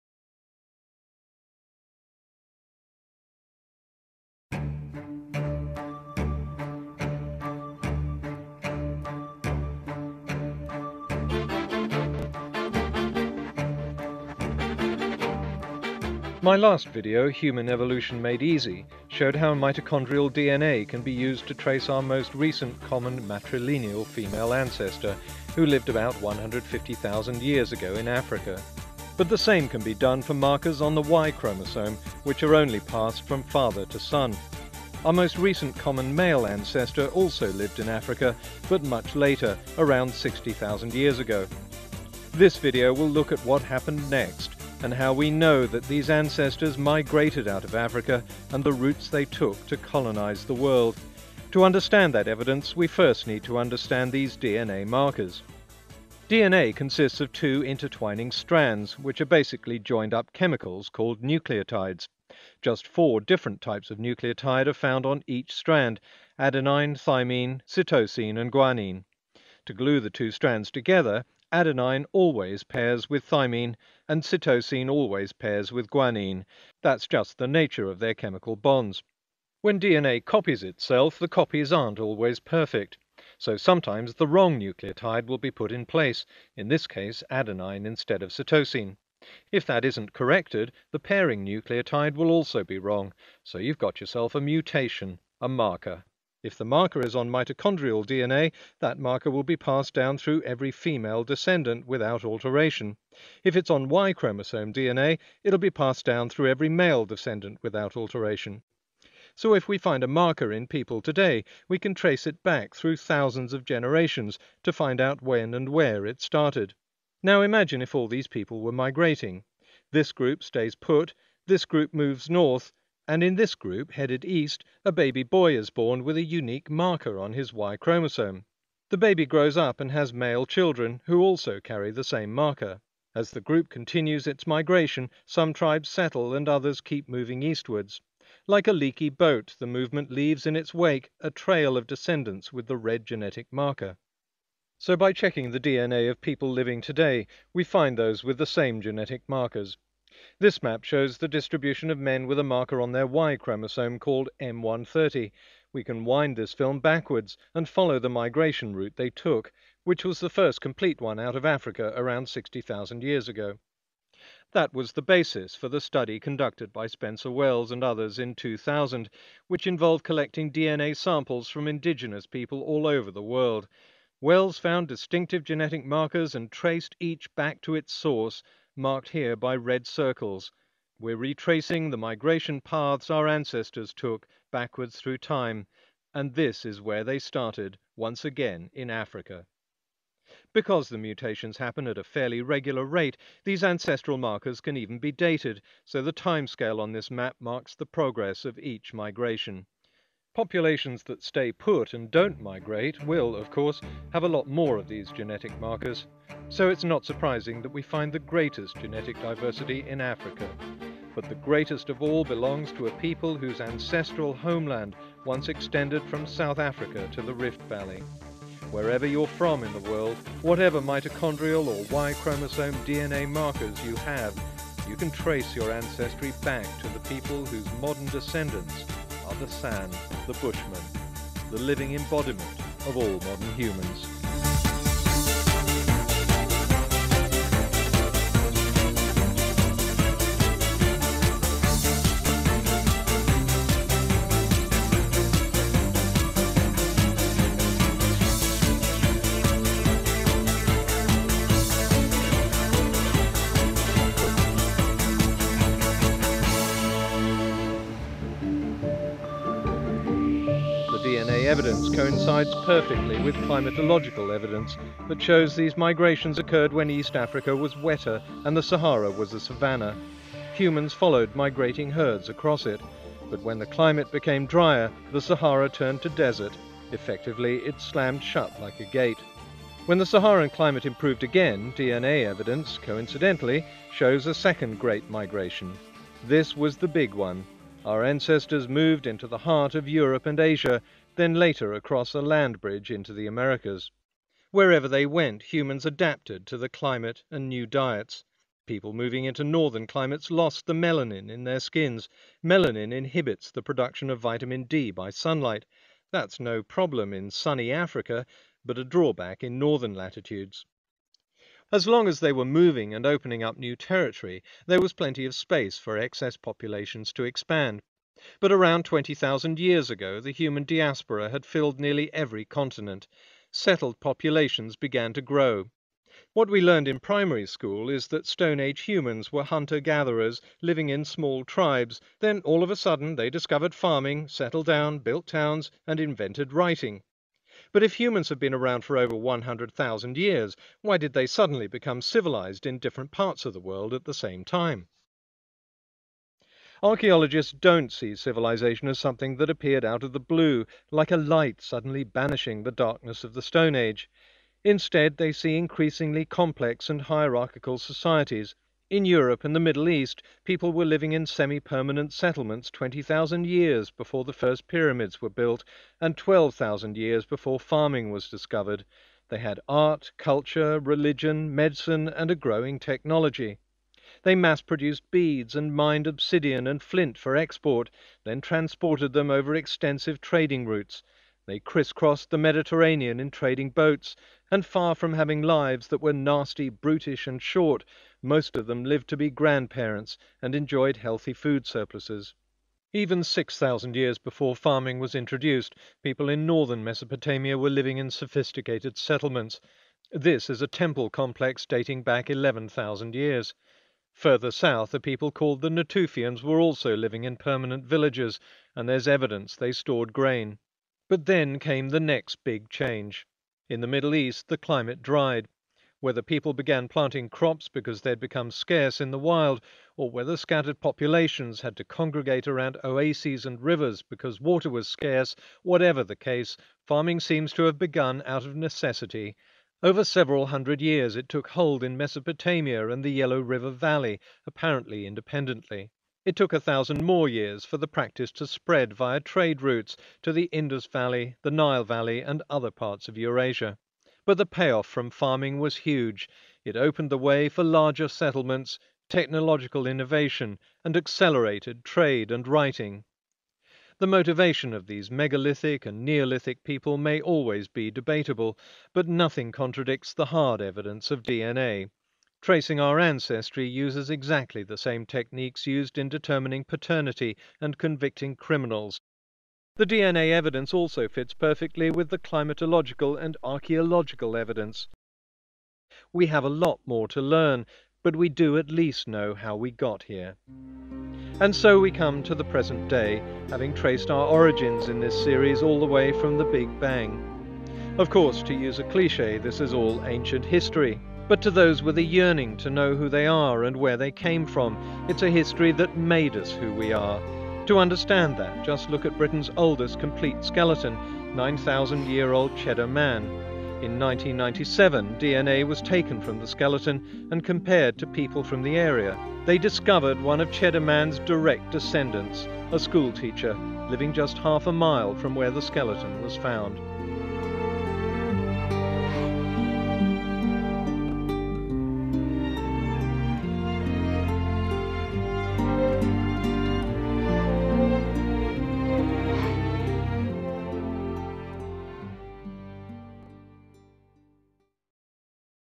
9 -- Human Ancestry Made Easy (for schools)
The scientific evidence for how humans migrated out of Africa. This is part nine of the  series that shows the evidence for our origins, starting with the Big Bang. This is a schools version of Human Ancestry Made Easy, with most of the material critical of creationism removed.

Does this video look better than the others? I uploaded a much higher quality  around 400MB instead of the usual 30.

For music creds, please see the video description in the adult version, Human Ancestry Made Easy.